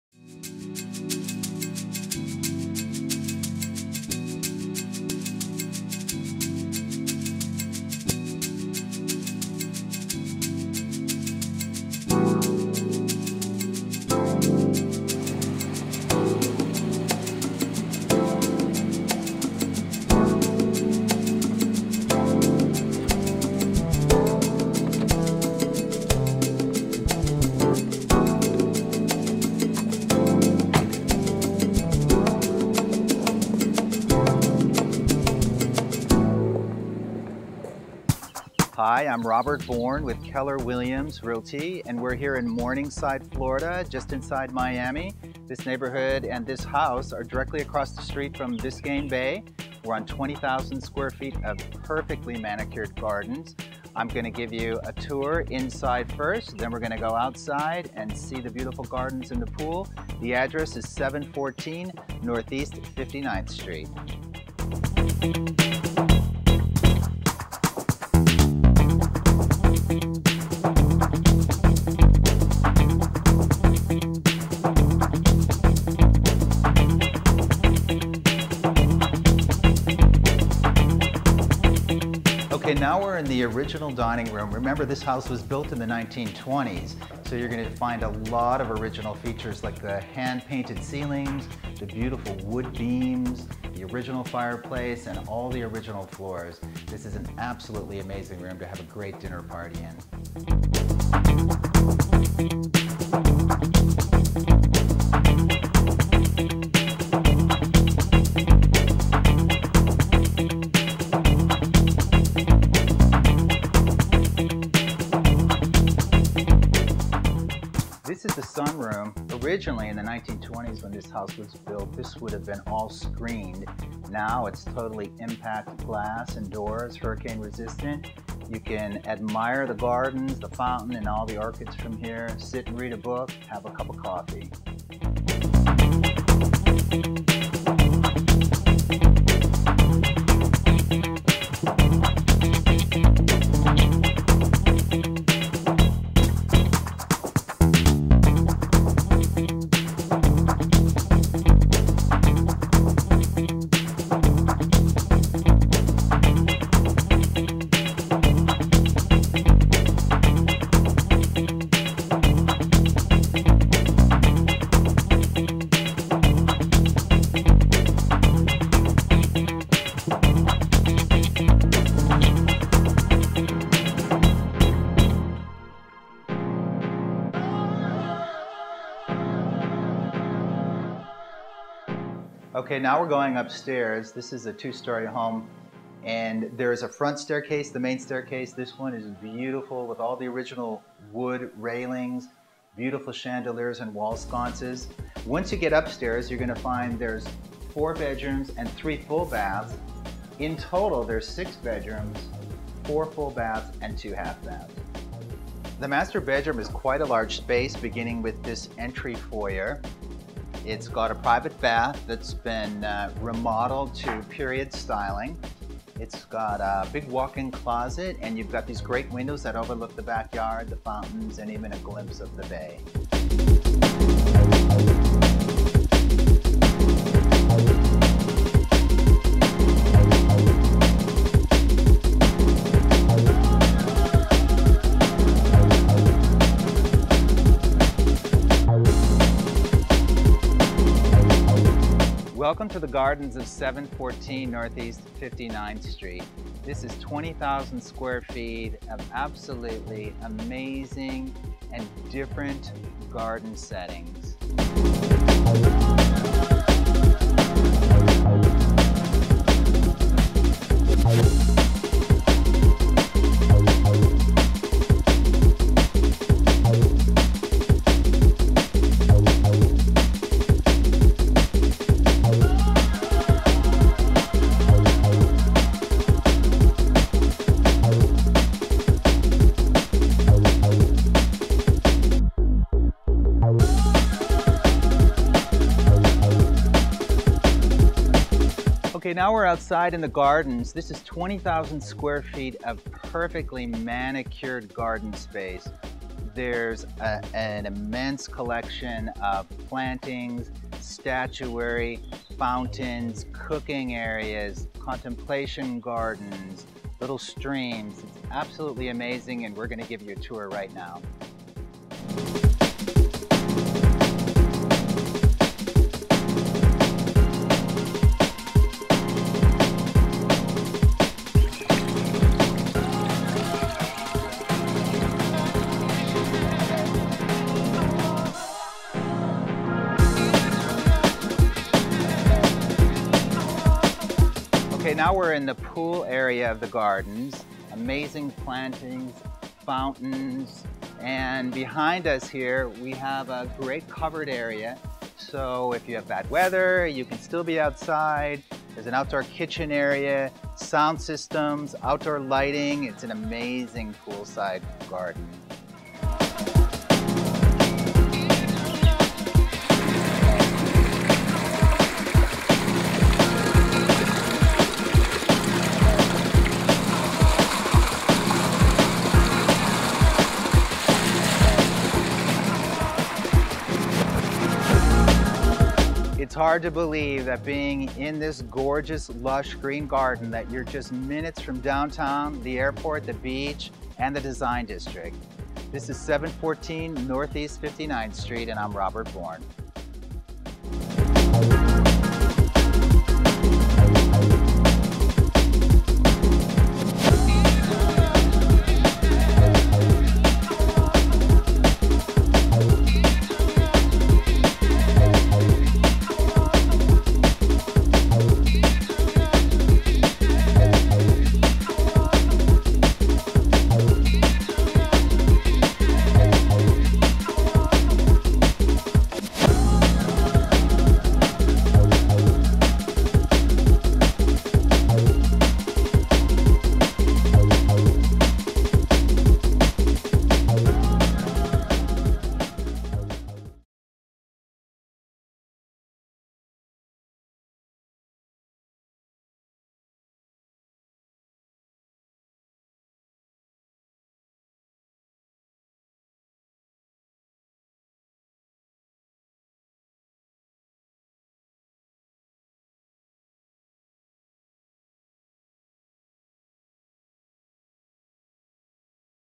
714 NE 59th Street, Morning Side - Luxurious Estate with Plush Gardens
LES JARDINS!  AMAZING 2 STORY MEDITERRANEAN VILLA IN GATED MORNINGSIDE WITH OVER 20,000 SQ. FT. OF ELEGANT, PERFECTLY MANICURED FORMAL EUROPEAN GARDEN SPACES.  6 BEDROOMS, 4 FULL BATHS & 2 HALF BATHS, POOL & 2 CAR GARAGE.  ELEGANT ENTRY FOYER, PAINTED/WOOD BEAMED CEILINGS, ORIGINAL WOOD & TILE FLOORS, FIREPLACE PLUS MANY MORE ORIGINAL FEATURES.  ALL BATHS RENOVATED, HUGE EAT-IN KITCHEN WITH SPACIOUS BUTLER'S PANTRY.  NEW ROOF, IMPACT WINDOWS & DOORS, NEW PLUMBING AND ELECTRIC.  3 ZONES CENTRAL A/C.